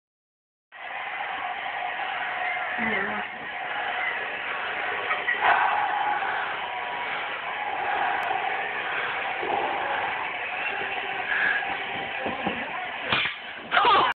Just playing the Xbox... Wells breaking light!
Wells breaking light playing xbox kenect.